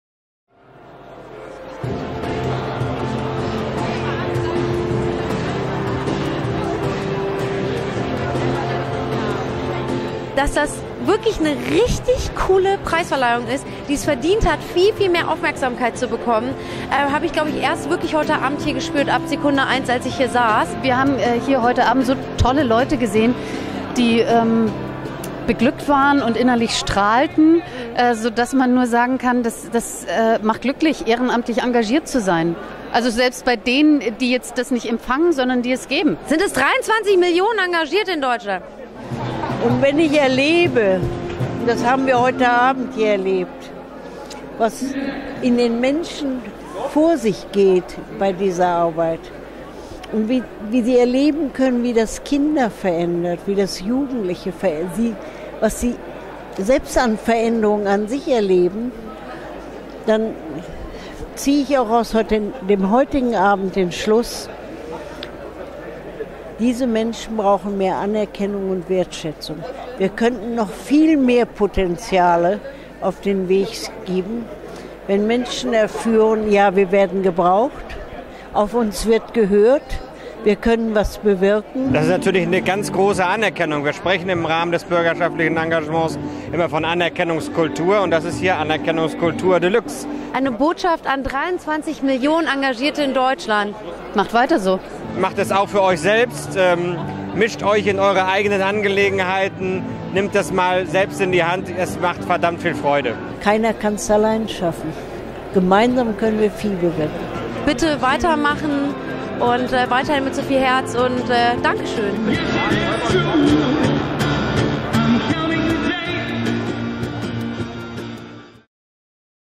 Verleihung Deutscher Bürgerpreis 2011
Der Film vermittelt über die Statements prominenter Gäste wie Bundestagspräsidentin a.D. Rita Süssmuth, MdB Sönke Rix, Partner des Deutschen Bürgerpreises, ZDF-Moderatorin Barbara Hahlweg und Publikumspreis-Laudatorin Nina Moghaddam einen Eindruck der Veranstaltung im ZDF Hauptstadtstudio Unter den Linden.